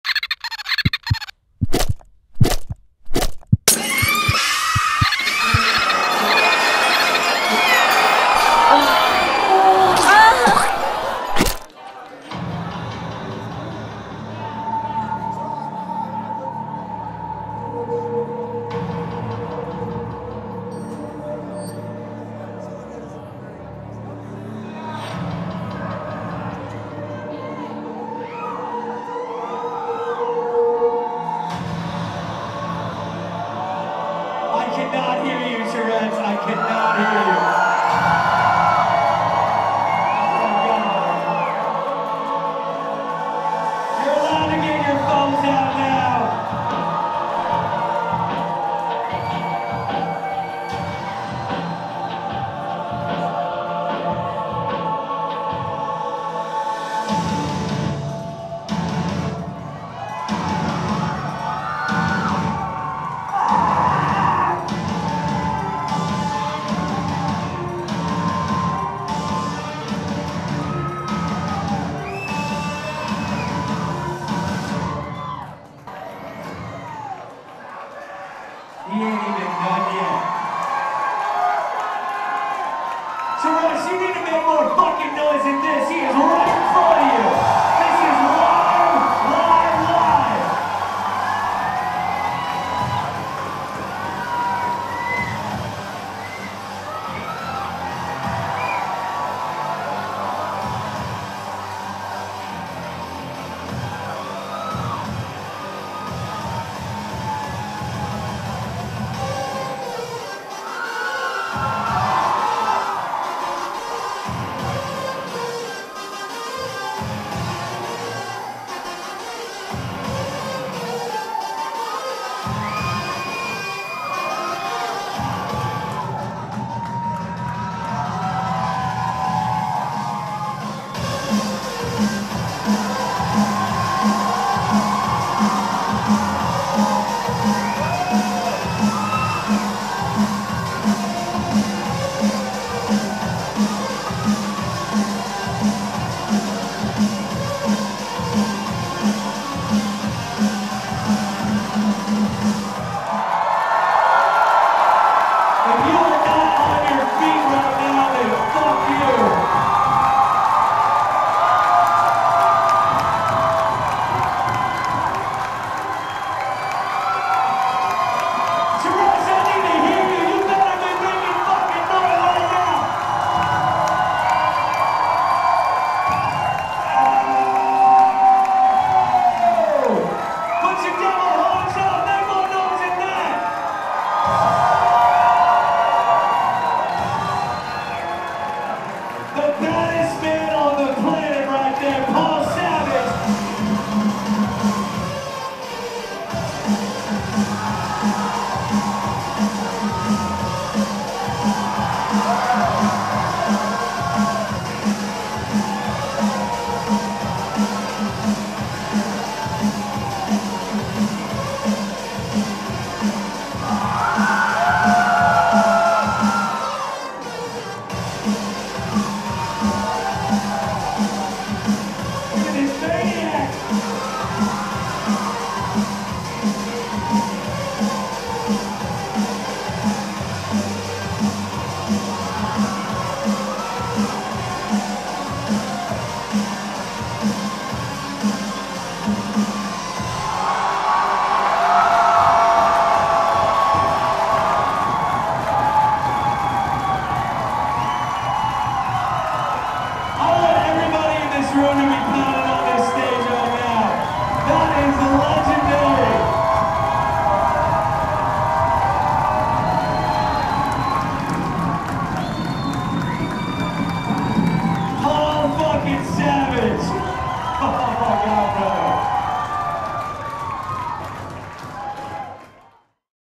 Paul Savage
Paul Savage, "the baddest man on the planet" performs his aerial act of pain with hooks in his back.